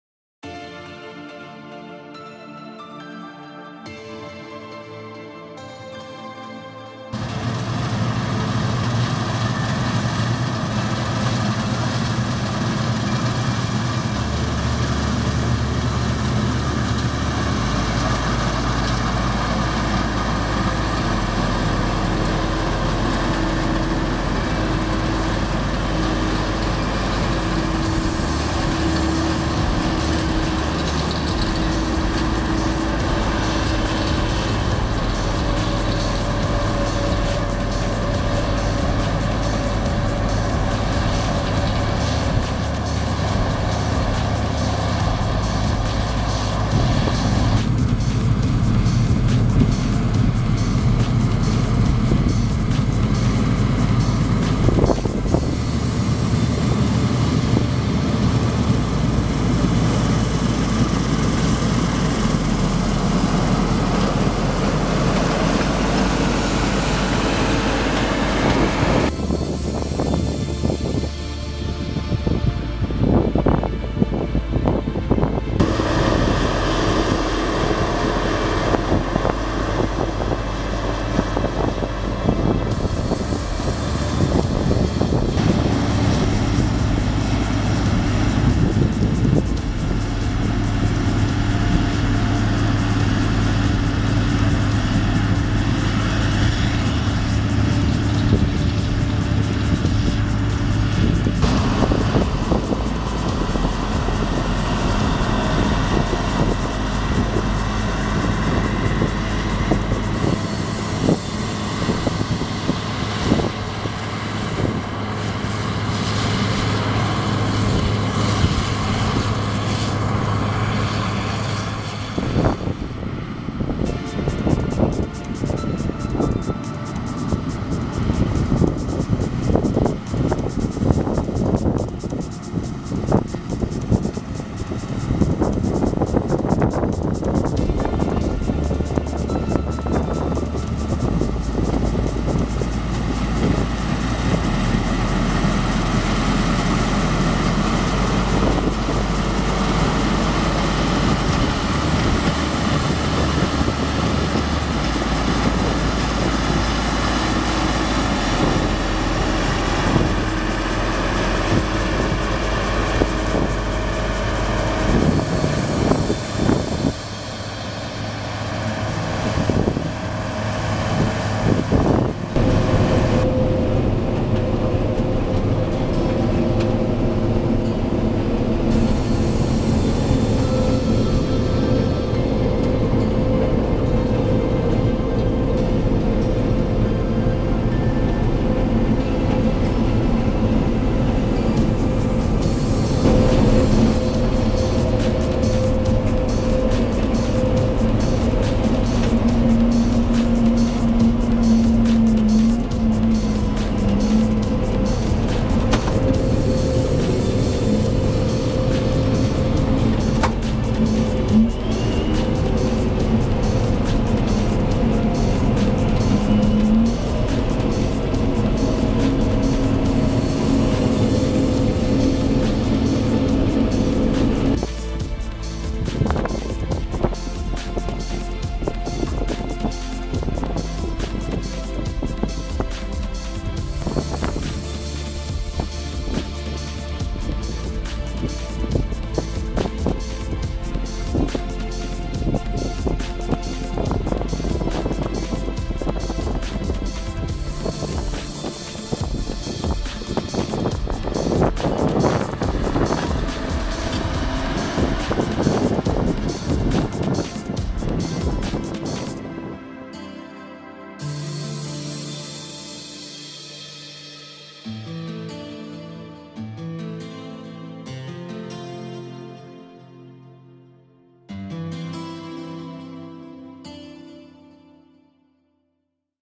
Gerste dreschen 2012 mit Claas Lexion 460
Gerste dreschen 2012 mit einem Claas Lexion 460. Abgefahren wurde mit einem Fendt 414 Vario und JCB 2170.